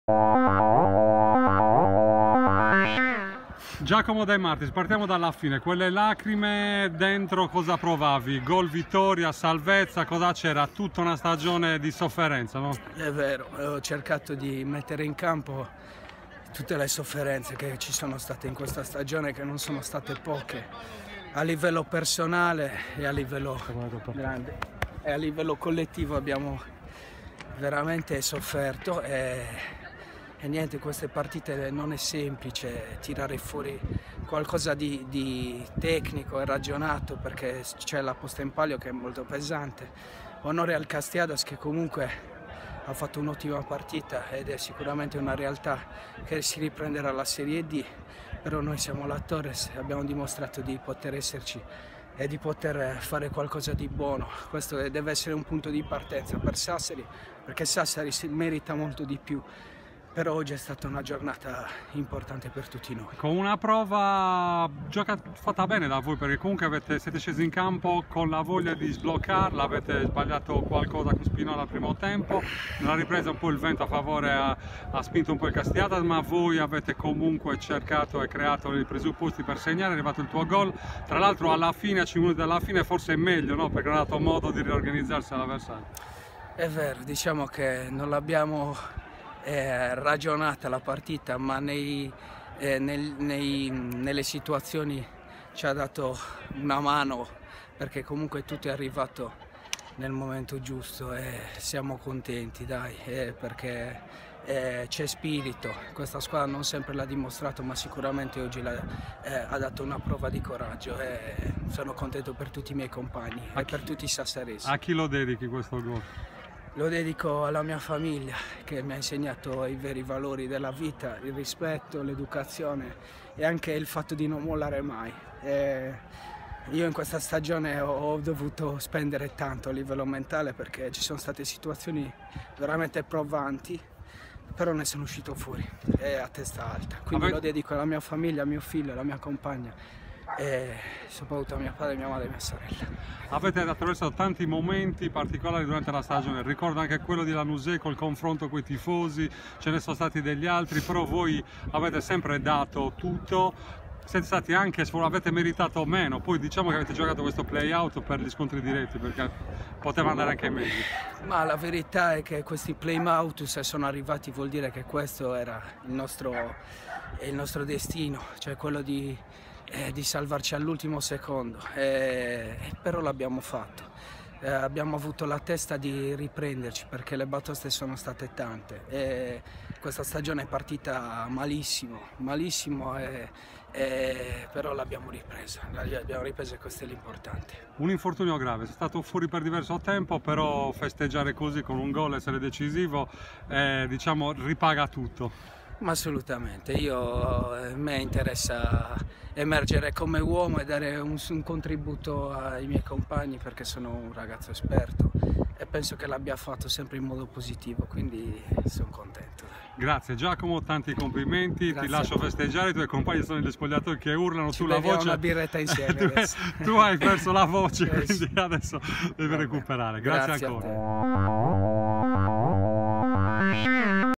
Giacomo Demartis Attaccante Torres 12-05-2019 - Diario Sportivo
Intervista a Giacomo Demartis, Attaccante Torres
Torres - Castiadas 1-0
Playout Serie D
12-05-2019